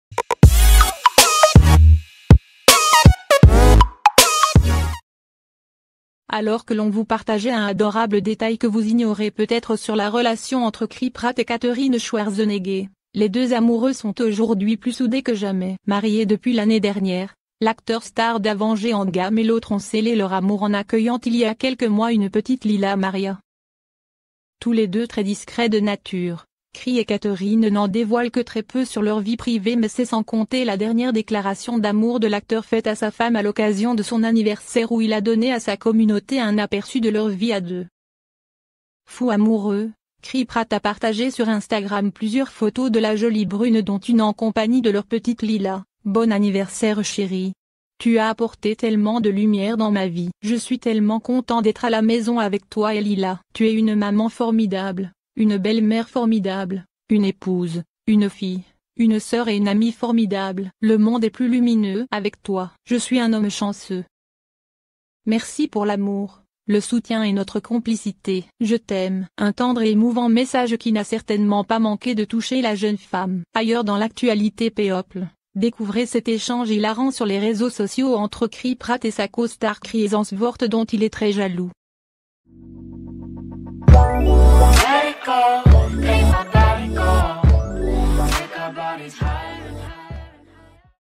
Chris Pratt (Avengers Endgame) marié à Katherine Schwarzenegger, il dévoile une rare photo de sa ...
Alors que l'on vous partageait un adorable détail que vous ignorez peut-être sur la relation entre Chris Pratt et Katherine Schwarzenegger, les deux amoureux sont aujourd'hui plus soudés que jamais. Mariés depuis l'année dernière, l'acteur star d'Avengers Endgame et l'auteure ont scellé leur amour en accueillant il y a quelques mois une petite Lyla Maria.

Tous les deux très discrets de nature, Chris et Katherine n'en dévoilent que très peu sur leur vie privée mais c'est sans compter la dernière déclaration d'amour de l'acteur faite à sa femme à l'occasion de son anniversaire où il a donné à sa communauté un aperçu de leur vie à deux.

Fou amoureux, Chris Pratt a partagé sur Instagram plusieurs photos de la jolie brune dont une en compagnie de leur petite Lyla : "Bon anniversaire chérie ! Tu as apporté tellement de lumière dans ma vie. Je suis tellement content d’être à la maison avec toi et Lyla. Tu es une maman formidable, une belle-mère formidable, une épouse, une fille, une sœur et une amie formidable. Le monde est plus lumineux avec toi. Je suis un homme chanceux.

Merci pour l'amour, le soutien et notre complicité. Je t'aime". Un tendre et émouvant message qui n'a certainement pas manqué de toucher la jeune femme.